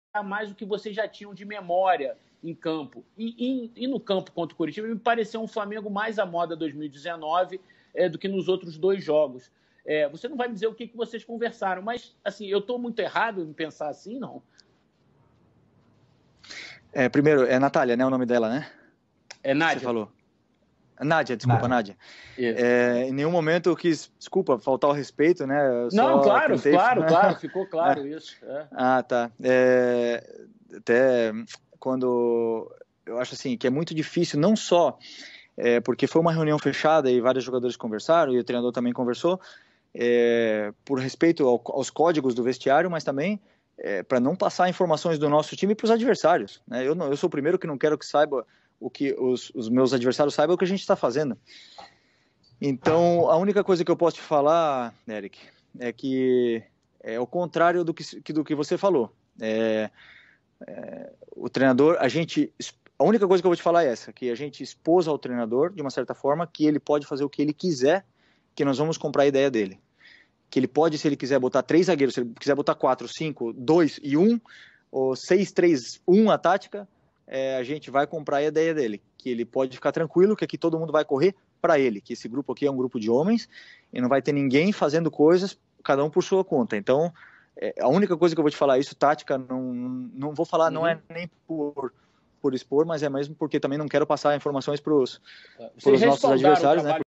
FELIPE LUIS DESMENTE REPÓRTER DA GLOBO e FAZ PASSAR VERGONHA AO VIVO ! APOIO INCONDICIONAL AO DOME
FELIPE LUIS DESMENTE REPÓRTER DA GLOBO E FAZ PASSAR VERGONHA AO VIVO ! APOIO TOTAL AO TREINADOR